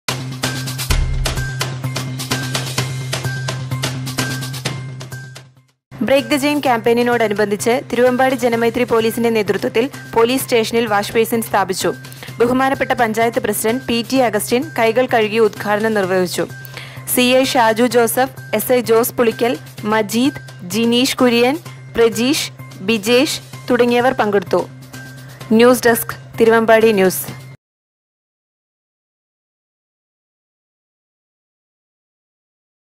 തിരുവമ്പാടി ജനമൈത്രി പോലീസ് സ്റ്റേഷനിൽ വാഷ്ബേസിനും സാനിറ്റെയ്സറും സ്ഥാപിച്ചു. - NEWS
തിരുവമ്പാടി : ബ്രേക്ക് ദ ചെയിൻ ക്യാമ്പയിനോടനുബന്ധിച്ച് തിരുവമ്പാടി ജനമൈത്രി പോലീസിന്റെ നേതൃത്വത്തിൽ പോലീസ് സ്റ്റേഷനിൽ വാഷ്ബേസിനും സാനിറ്റെയ്സറും സ്ഥാപിച്ചു.

തിരുവമ്പാടി ഗ്രാമപഞ്ചായത്ത് പ്രസിഡൻറ് പി ടി അഗസ്റ്റിൽ കൈകൾ കഴുകി ഉദ്ഘാടനം നിർവഹിച്ചു.

സി ഐ ഷാജു ജോസഫ് , എസ് ഐ ജോയി പുളിക്കൽ, മജീദ്, ജിനീഷ് കുര്യൻ, പ്രജീഷ്, ബിജേഷ്, തുടങ്ങിയവർ പങ്കെടുത്തു.